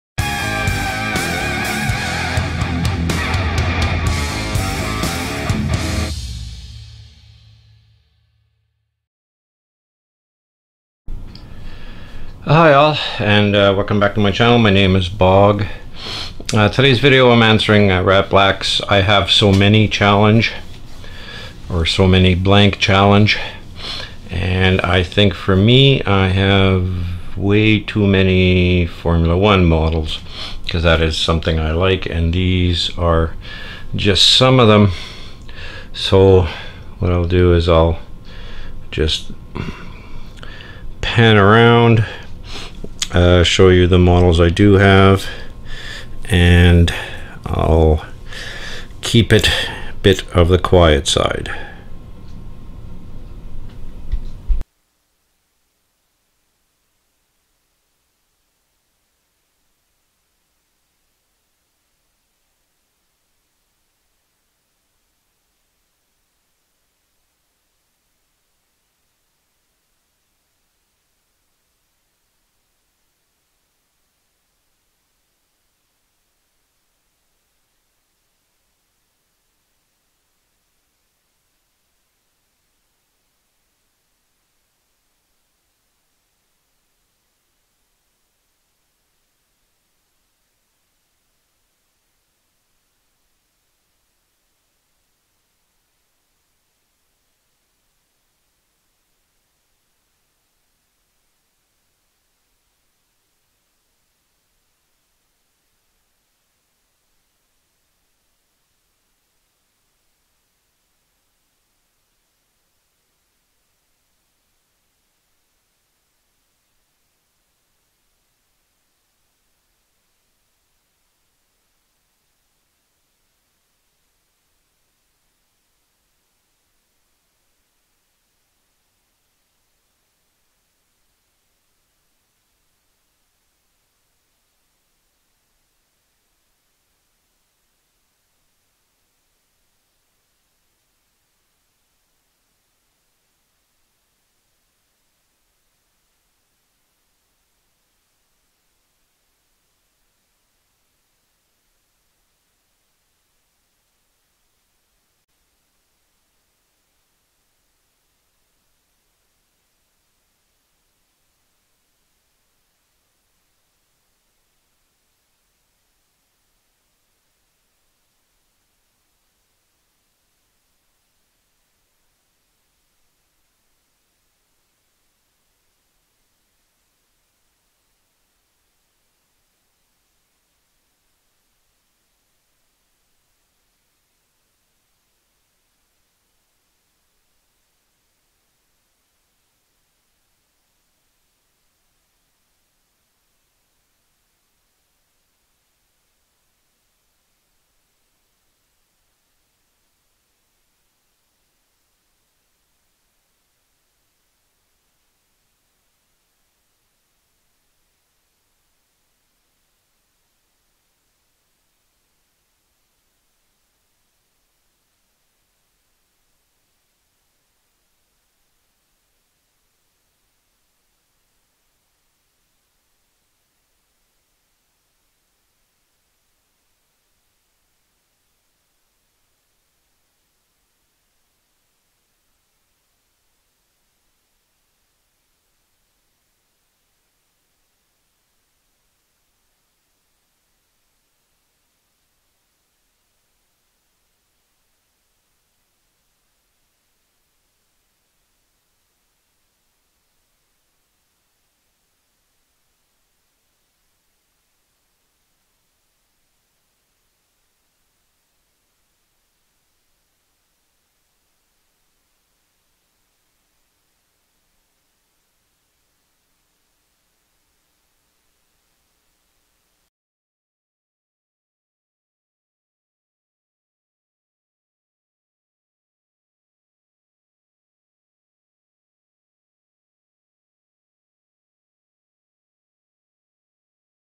ratblacks_diecast - I HAVE SO MANY_________ CHALLENGE
Hi all. I think I have lots of Formula 1 Models, what do you think. Enjoy.